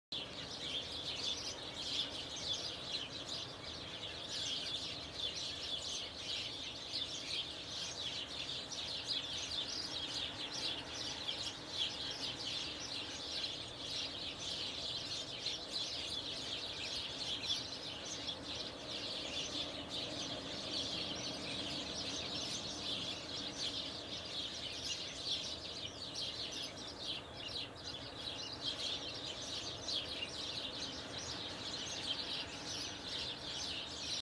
My goldie Movie
just to show how much niger seed gets wasted.